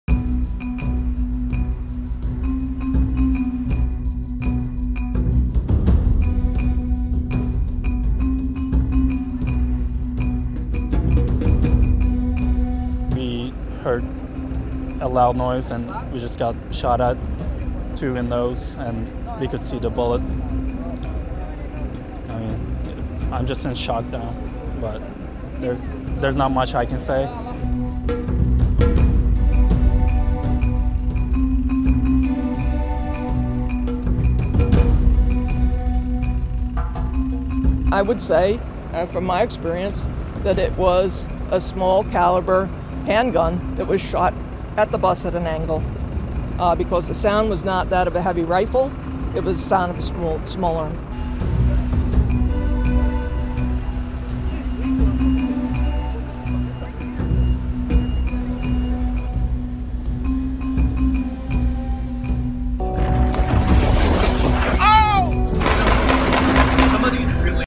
Rio Olympics media bus attacked on its way to Barra Olympic park
A bus carrying Rio Olympics journalists was attacked on Tuesday.
It was not clear whether the attacks were made by stones or bullets.

Around 12 people were in the bus when the attack was made. A Belarus journalist suffered a cut hand in the incident and two others suffered minor injuries.

The bus was later escorted to safety with the help of the military.